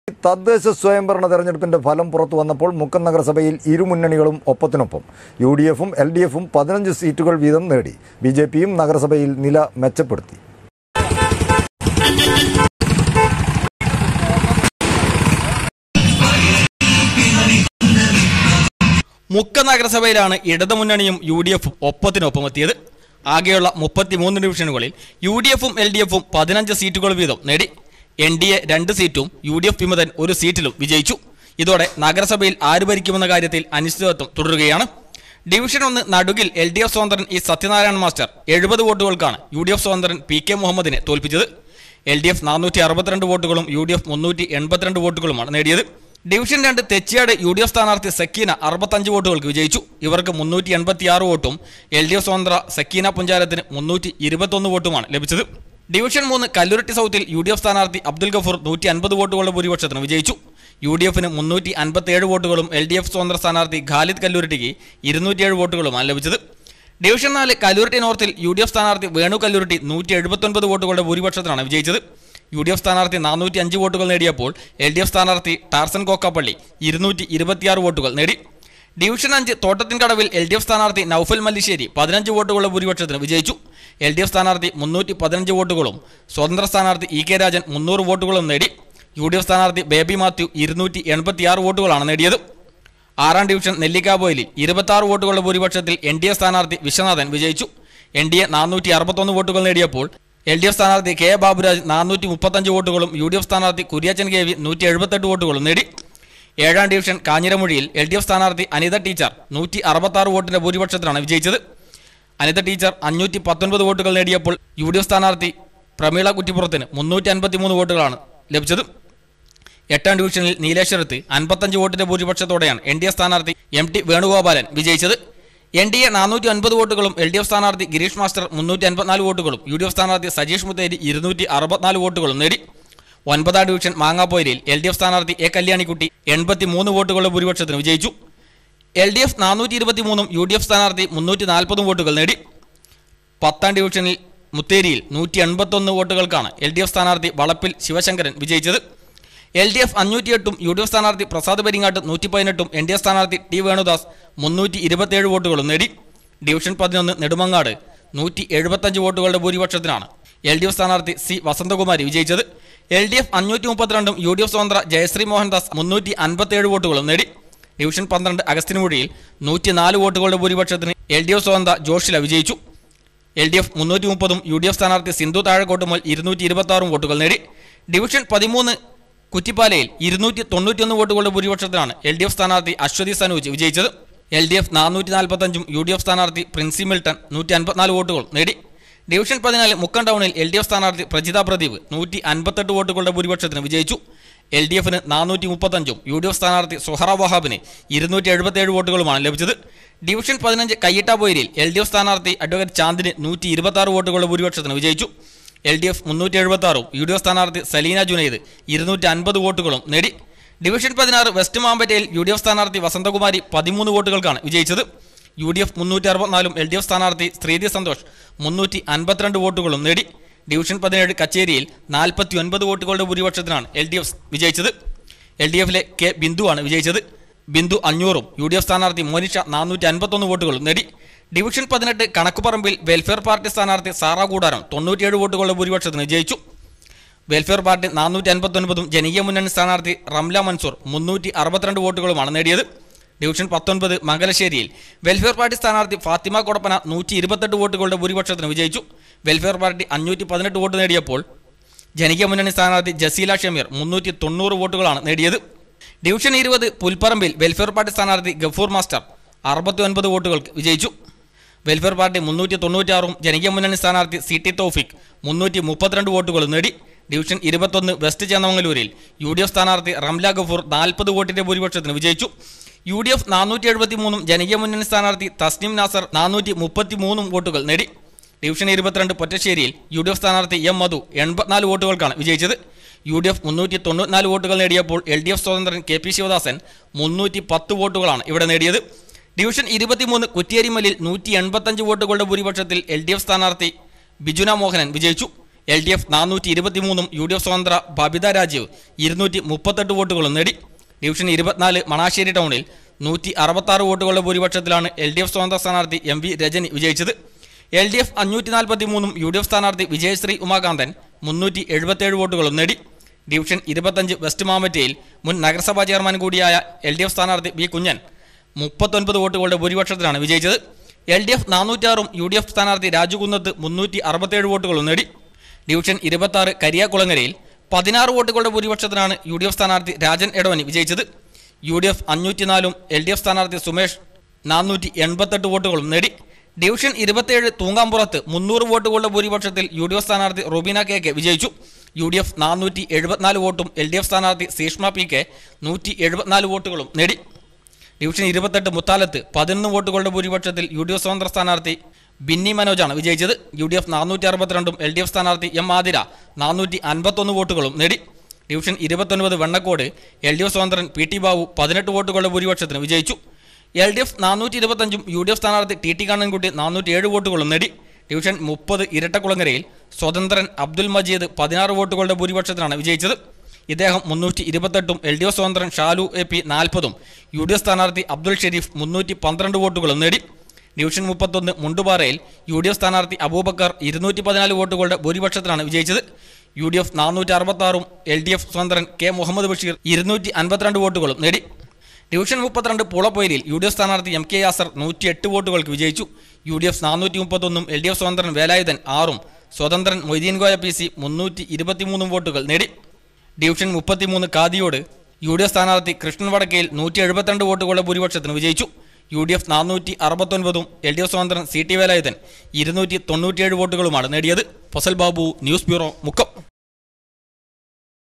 തദ്ദേശ സ്വയംഭരണ തെരഞ്ഞെടുപ്പിൻ്റ ഫലം പുറത്ത് വന്നപ്പോൾ മുക്കം നഗരസഭയിൽ ഇരു മുന്നണികളും.
തദ്ദേശ സ്വയംഭരണ തെരഞ്ഞെടുപ്പിൻ്റ ഫലം പുറത്ത് വന്നപ്പോൾ
മുക്കം നഗരസഭയിൽ ഇരു മുന്നണികളും ഒപ്പത്തിനൊപ്പം. യു ഡി എഫും എൽ ഡി എഫും 15 സീറ്റുകൾ വീതം നേടി. ബി ജെ പിയും നഗരസഭയിൽ നില മെച്ചപ്പെടുത്തി